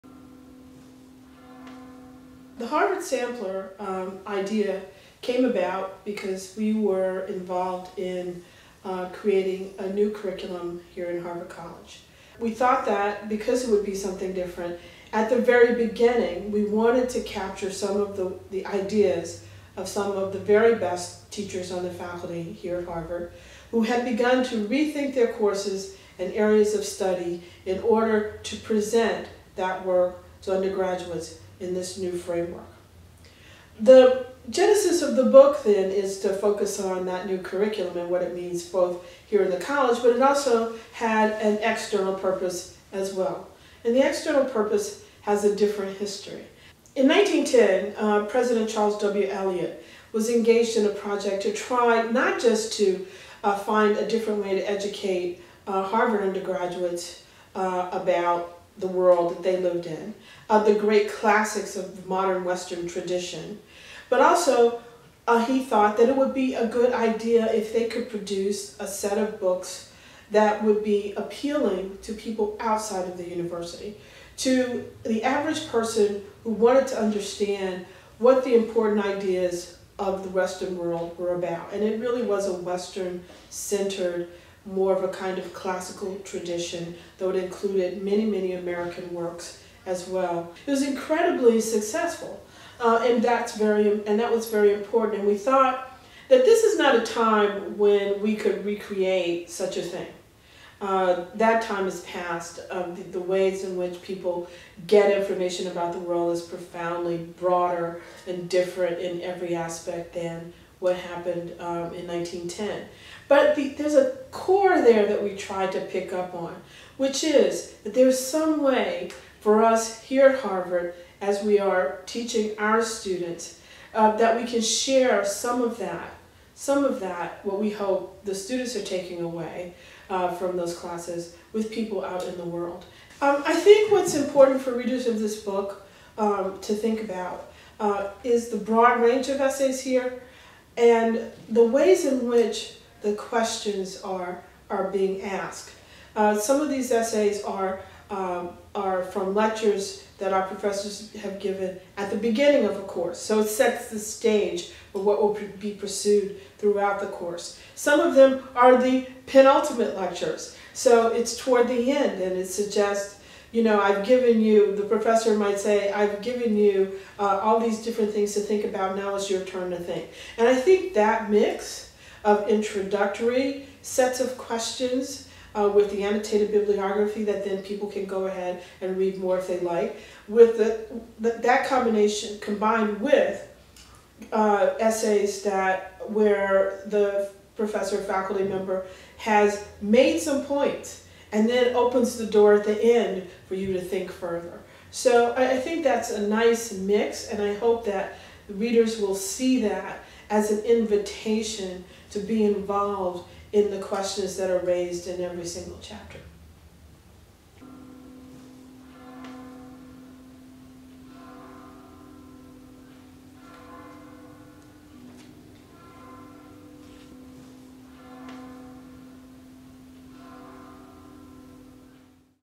The Harvard Sampler – Evelynn M. Hammonds
Harvard College Dean Evelynn M. Hammonds explains The Harvard Sampler: Liberal Education for the Twenty-First Century, a new collection of essays sampling topics at the forefront of academia in the twenty-first century.

Written by faculty members at the cutting edge of their fields, the essays in The Harvard Sampler offer a clear and accessible overview of disciplines that are shaping the culture, and even the world. Participating faculty include Ali S. Asani, Lawrence Buell, Harry R. Lewis, Jonathan B. Losos, Charles S. Maier, Karin B. Michels, Steven Pinker, Mathias Risse, T. M. Scanlon, John H. Shaw, Werner Sollors, and Laurel Thatcher Ulrich.